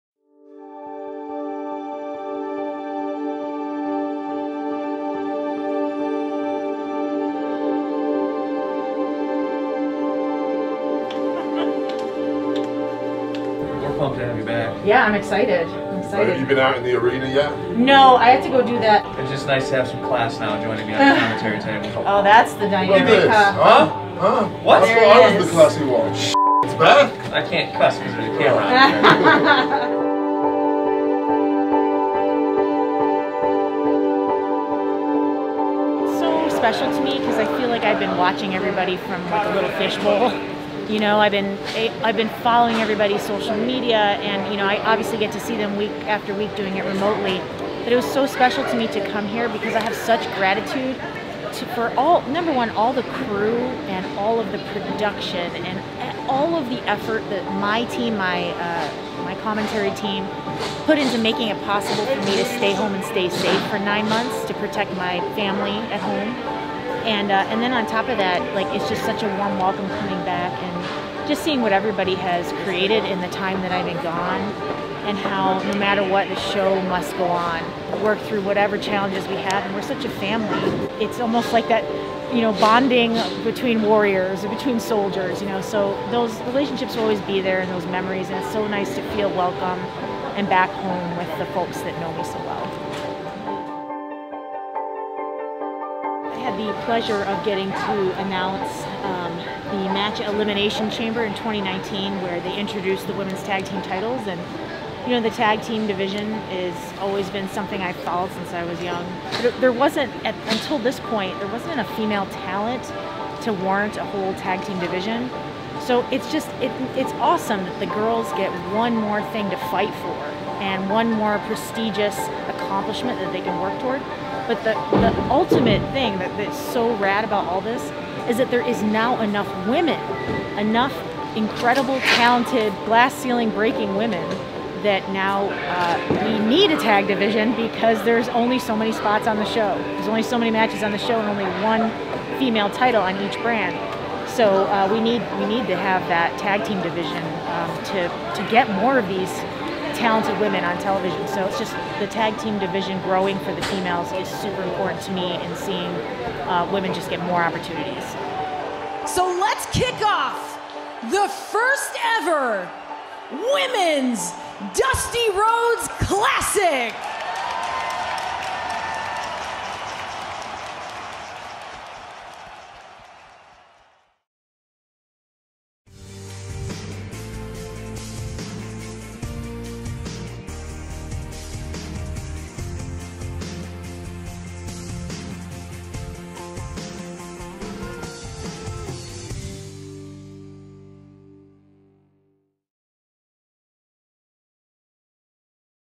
We Missed You, Beth Phoenix!
After spending nine months with her family, Beth Phoenix reflects on her return to the Capitol Wrestling Center and NXT! The WWE Hall of Famer talks about the importance of the first-ever Women’s Dusty Classic and why the ladies in WWE deserve more.